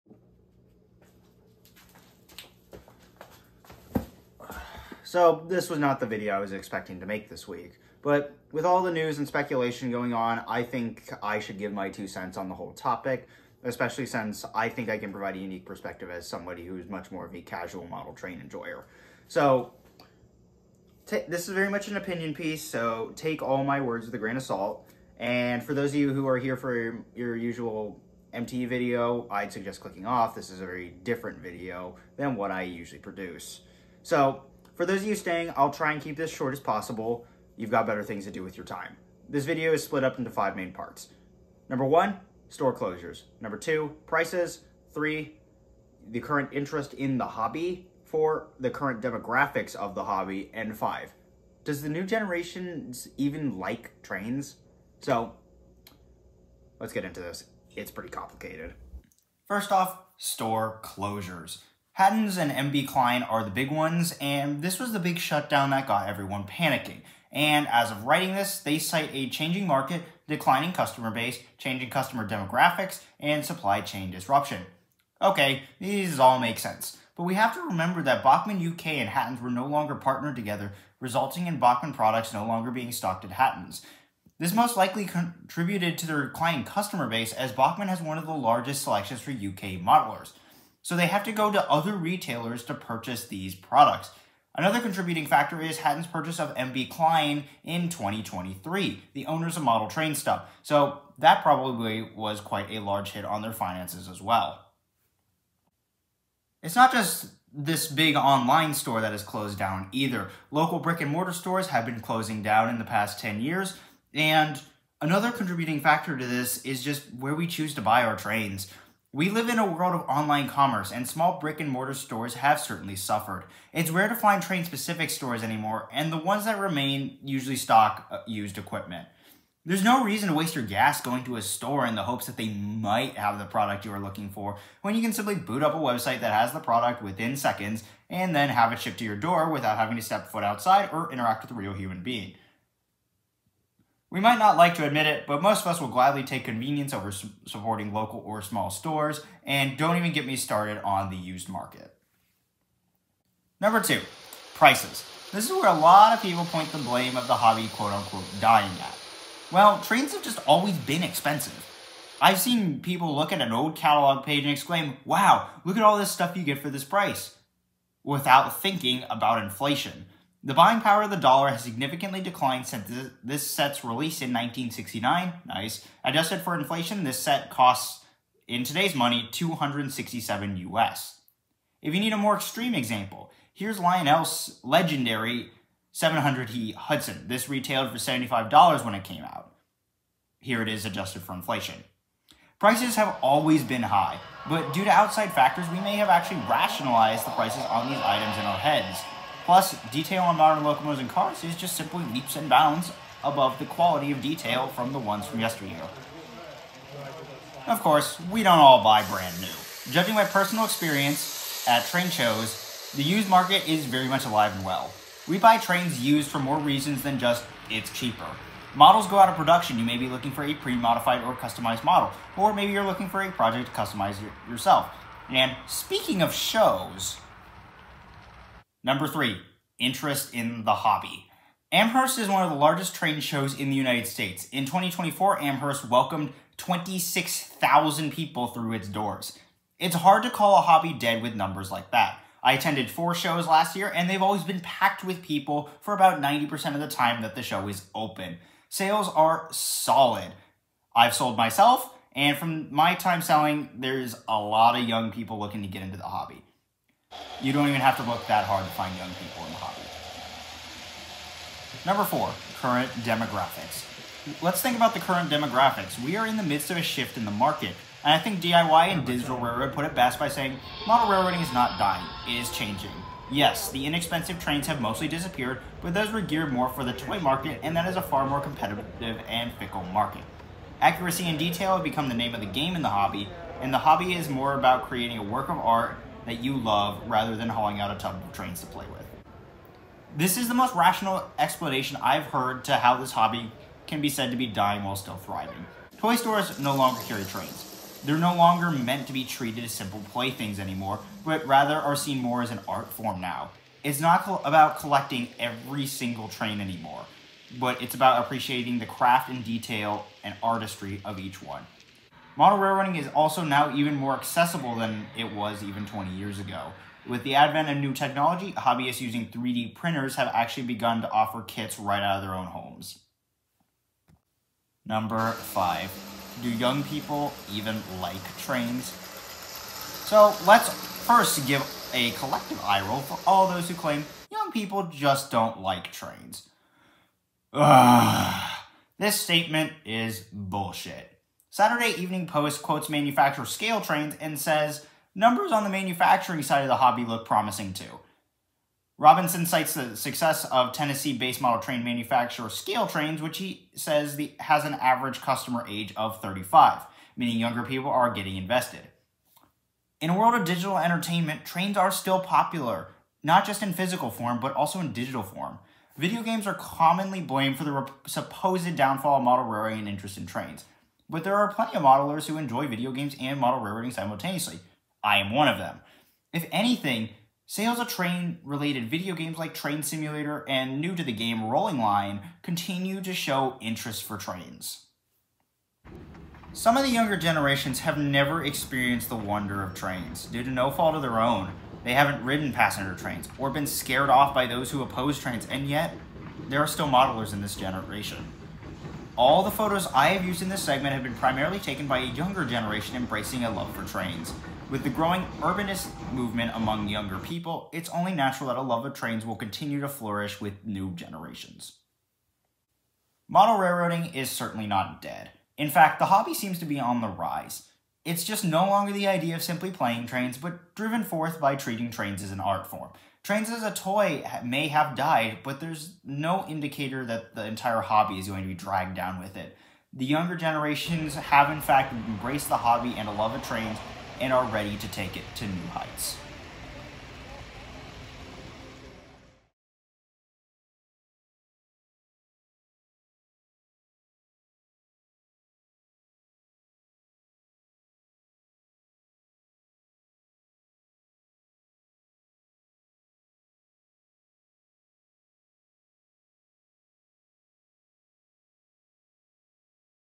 Model railroading isn't dead. Stop panicking.
Ugggh. Can't believe a single store closing signifies "the dark age being upon us" or some bullshit.